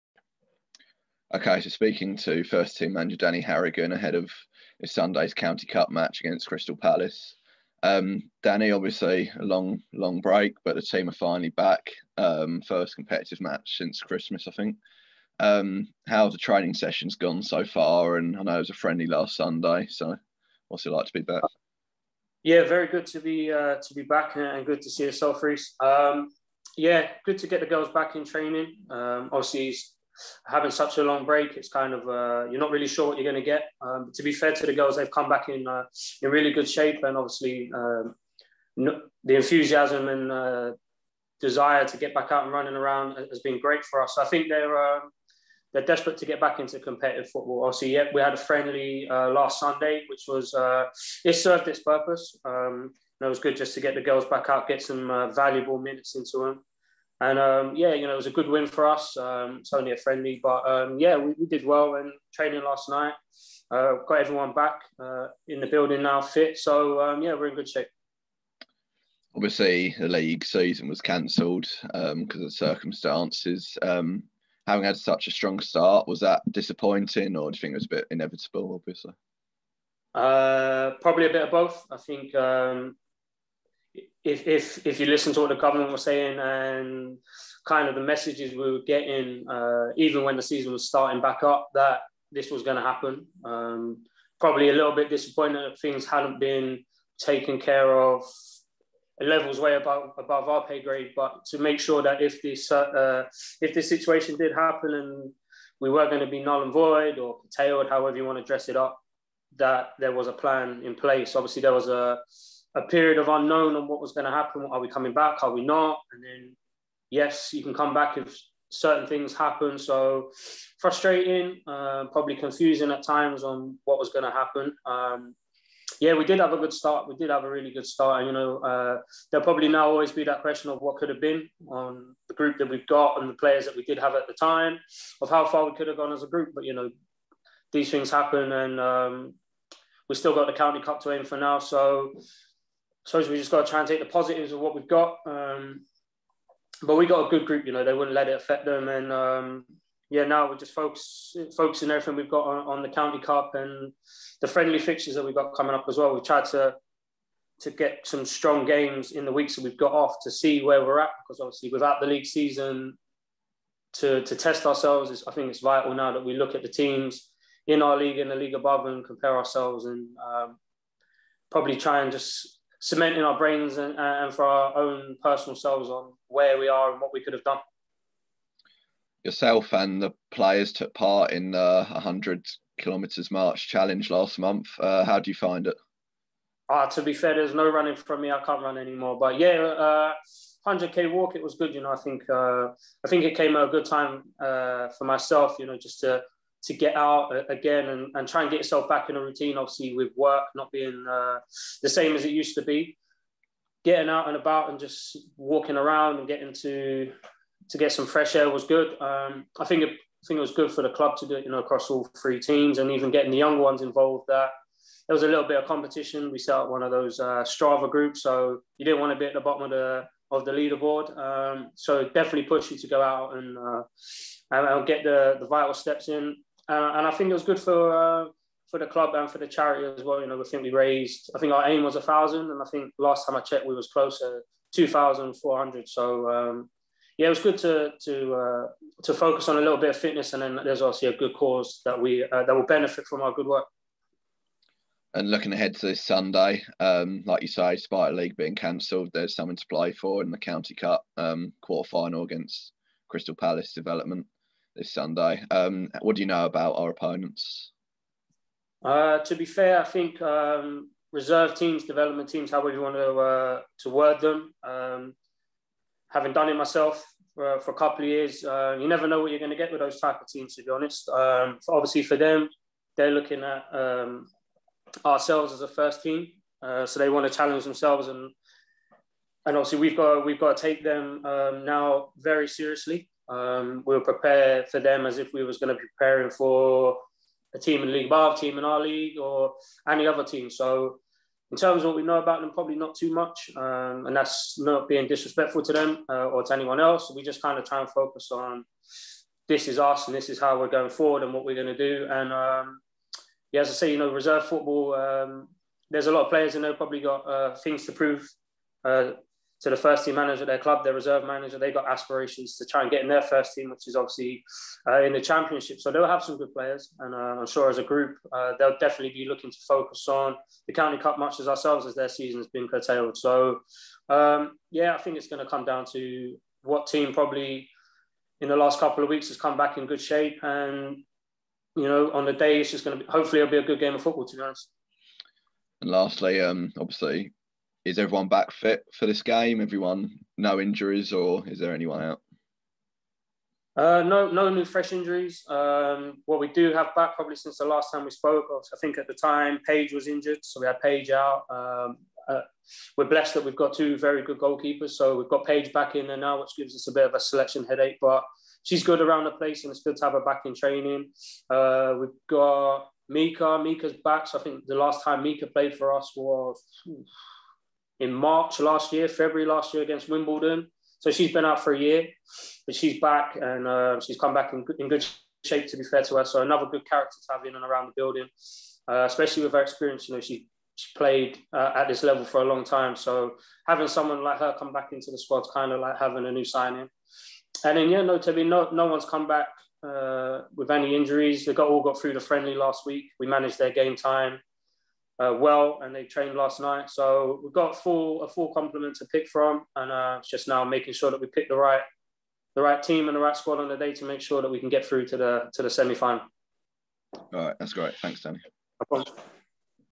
MANAGER INTERVIEW | Crystal Palace Development (H)
First team boss Danny Harrigan speaks about the 5 month break from football and looks ahead to this Sunday's match against Crystal Palace Development in the County Cup Quarter Final.